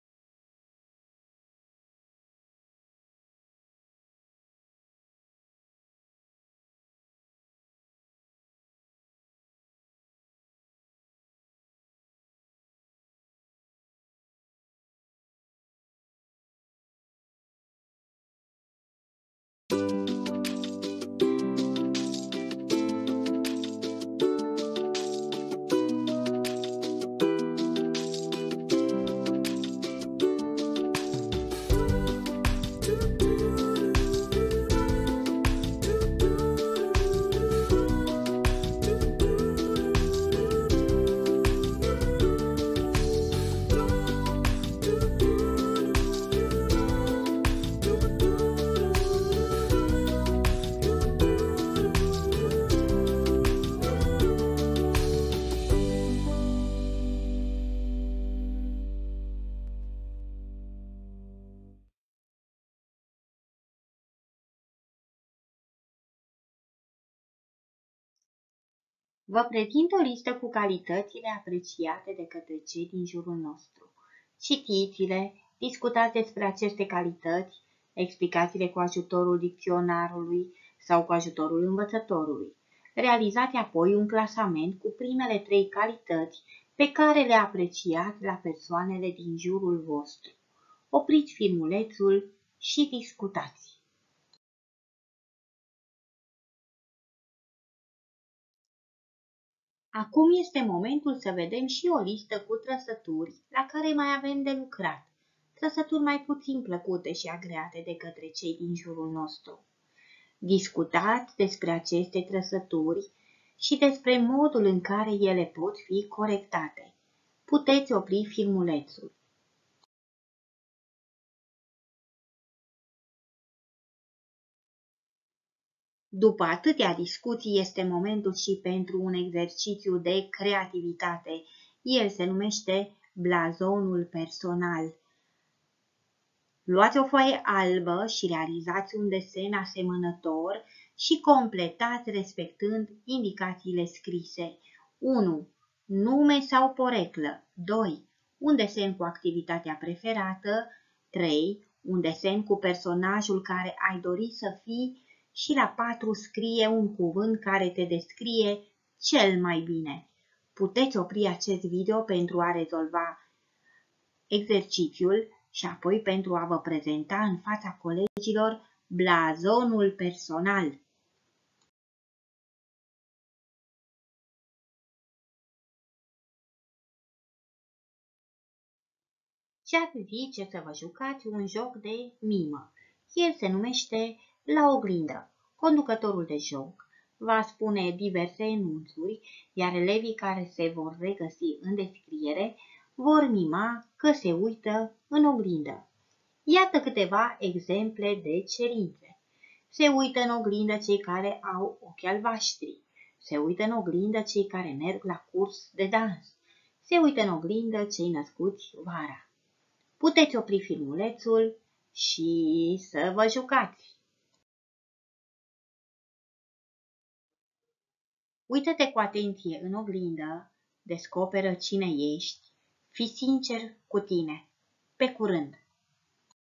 În oglindă
Resursa este un video din Power Point. Elevii sunt invitați
să se descrie, să discute despre trăsăturile pozitive/negative grupate într-un tabel, să își exprime opinia cu privire la modul de corectare a unor defecte.
Pentru prezentarea propriei persoane elevii vor alcătui ”Blazonul personal” și vor participa la un joc de mimă ”Se uită în oglindă”.

Autor: Elisabeta Maria Mînecuță